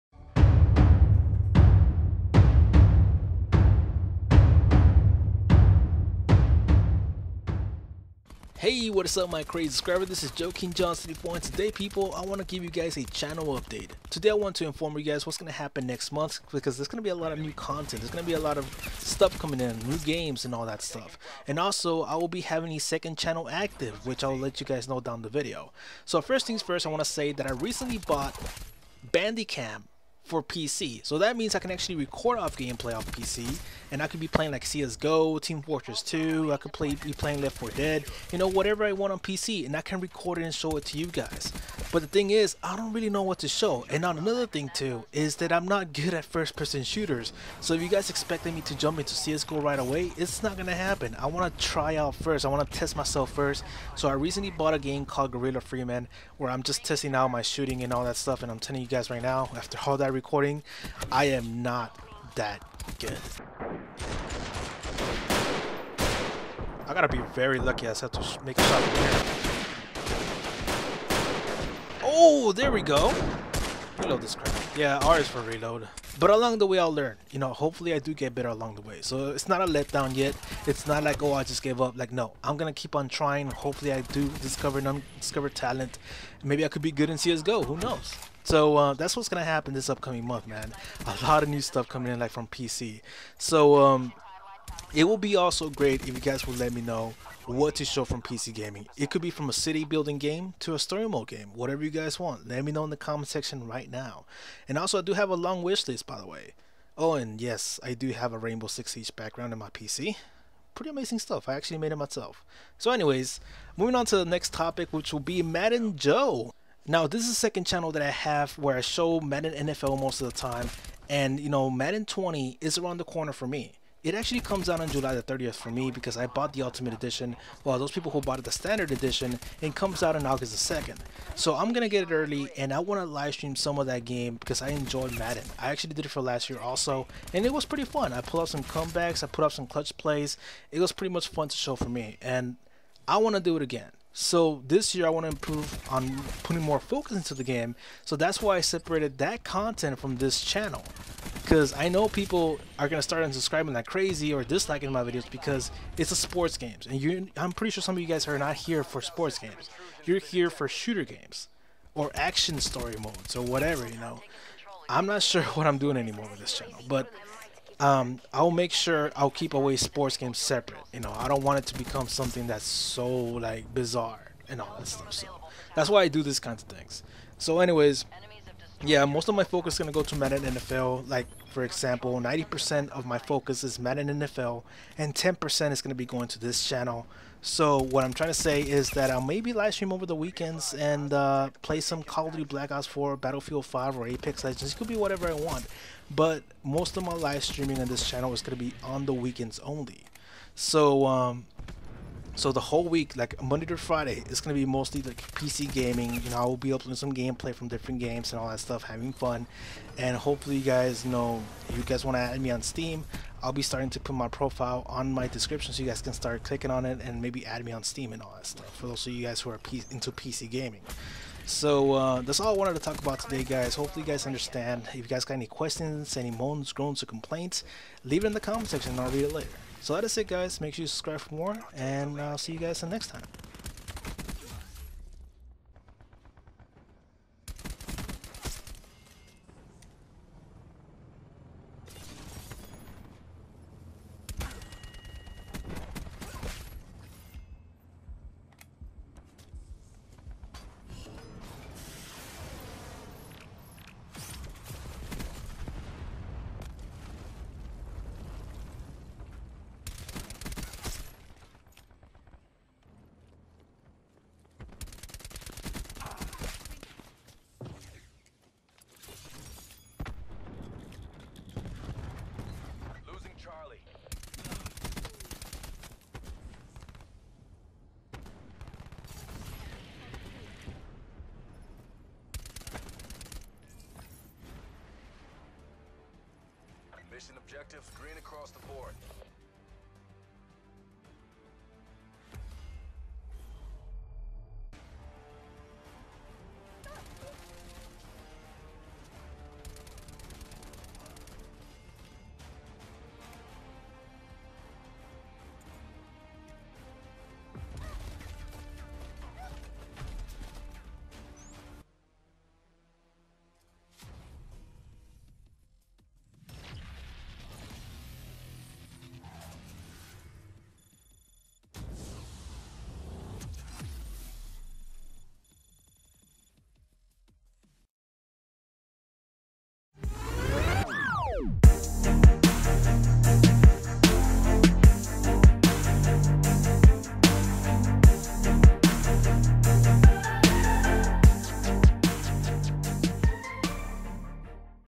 Channel Update! Content Changes + PC Gaming and Madden 20
Today I will give you guys an update on my future content on this channel. It includes PC gaming talks and my 2nd channel with Madden 20.